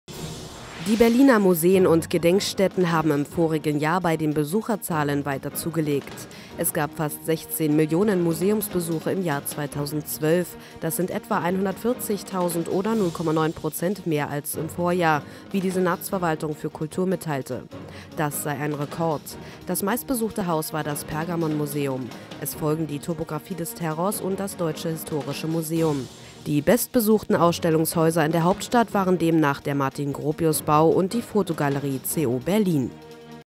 Besucherzahlen bei Berliner Museen gestiegen
Die Berliner Museen und Gedenkstätten haben im vorigen Jahr bei den Besucherzahlen weiter zugelegt. Es gab fast 16 Millionen Museumsbesuche im Jahr 2012 - das sind etwa 140 000 oder 0,9 Prozent mehr als im Vorjahr, wie die Senatsverwaltung für Kultur mitteilte. Das sei ein Rekord. Das meistbesuchte Haus war das Pergamonmuseum. Es folgen die Topographie des Terrors und das Deutsche Historische Museum. Die bestbesuchten Ausstellungshäuser in der Hauptstadt waren demnach der Martin-Gropius-Bau und die Fotogalerie C/O Berlin.